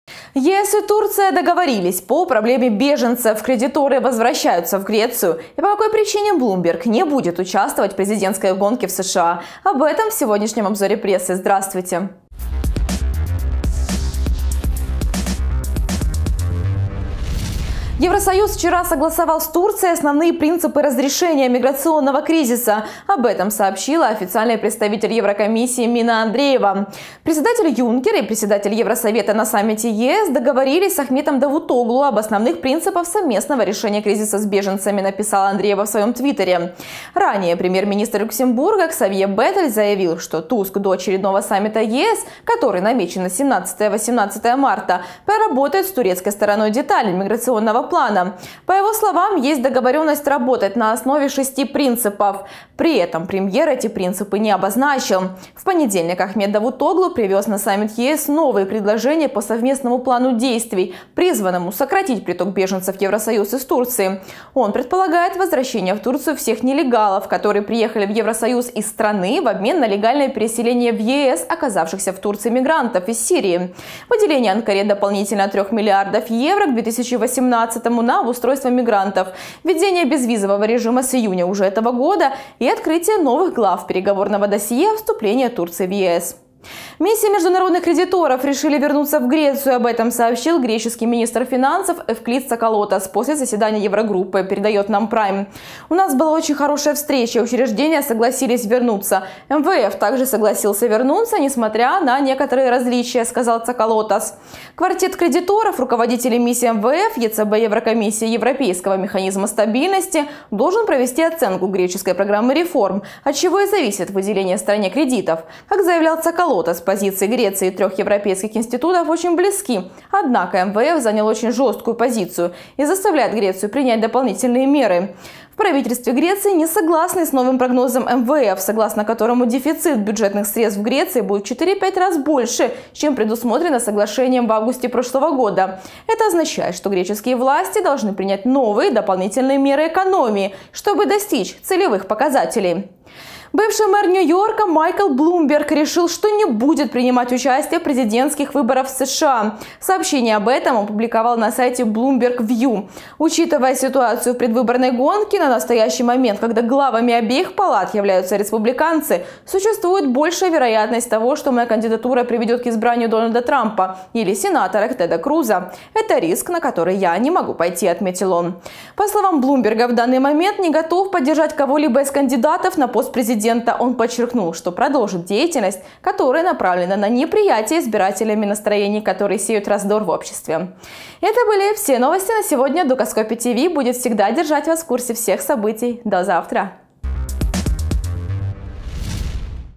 Кредиторы возвращаются в Грецию
ЕС и Турция договорились по проблеме беженцев;
Блумберг не будет участвовать в президентской гонке в США